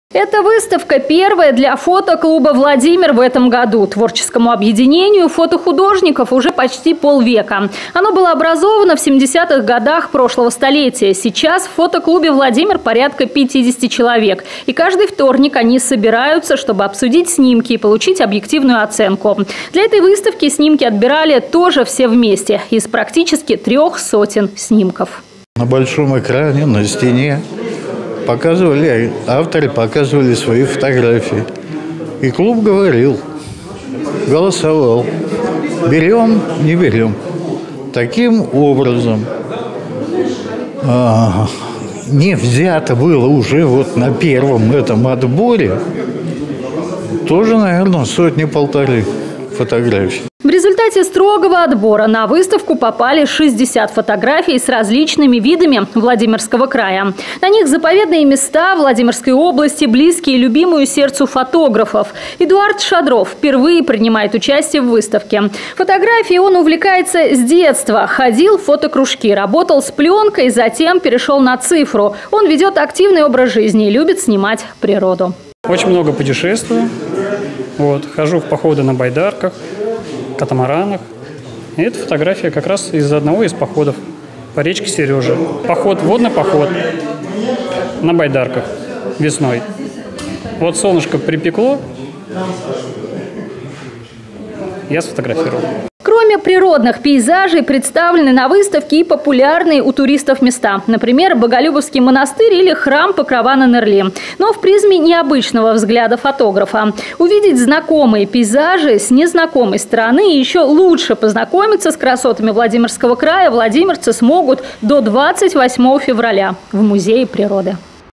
Фотовыставка - заповедные уголки
В музее природы открылась фотовыставка "Заповедные места Владимирской области".

На ней предсавлены работы участников фотоклуба "Владимир", показывающие красоту нашего региона.